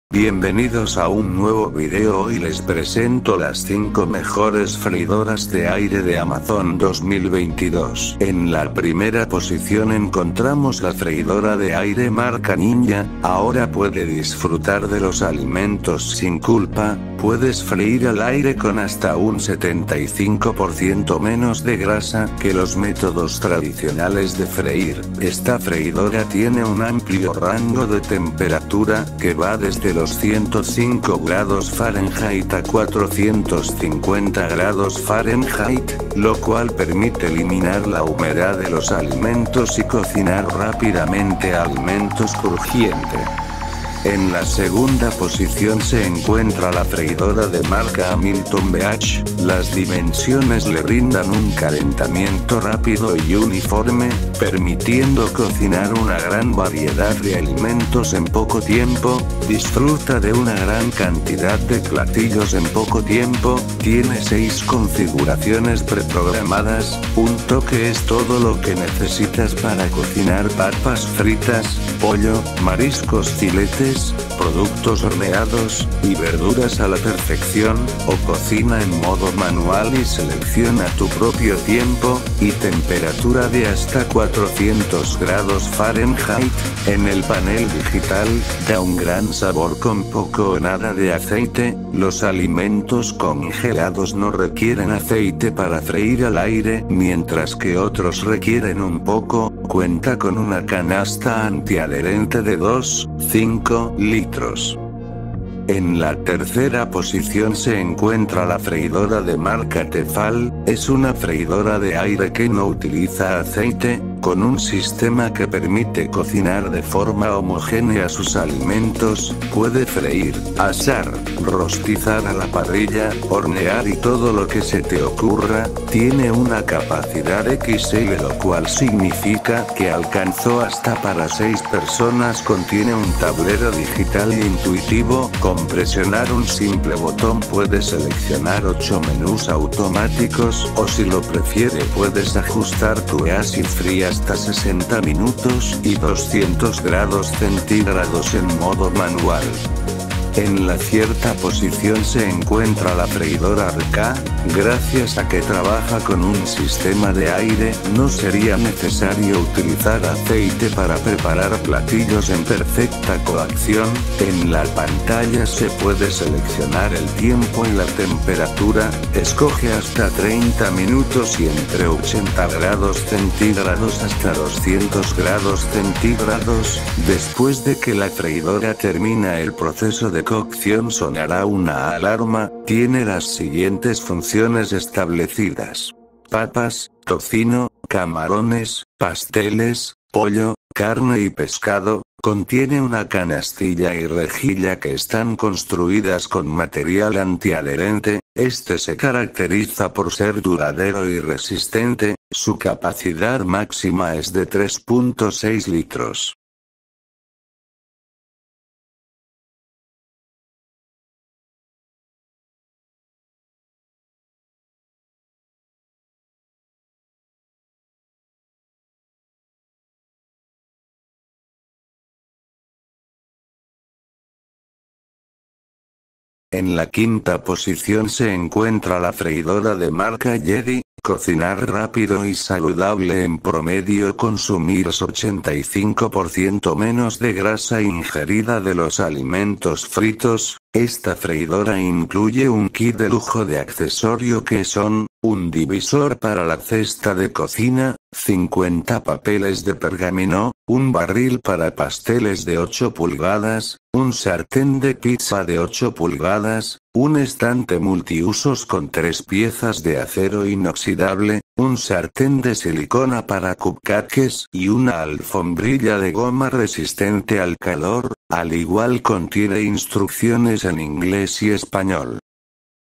✅Las 5 mejores freidoras de aire (freidora sin aceite) de Amazon 2022✅
1.-Ninja - Freidora de aire, Negro/Gris, 3,78L, 1, 4.9

2.-Hamilton Beach 35051 Freidora de Aire Digital con Capacidad de 2.5 Litros, Canasta Antiadherente, Porciones para 2-3 Personas, 6 Configuraciones, Temporizador de 60 Minutos, Hasta 204.4 °C

3.-T-Fal EY4018MX Freidora sin aceite EASY FRY 4.2L

4.-RCA Freidora de Aire Digital de 3.6 Litros RC-106

5.-Yedi Paquete Total Freidora de Aire, 4 Litros, Kit de Accesorios de Lujo, Recetas, Negro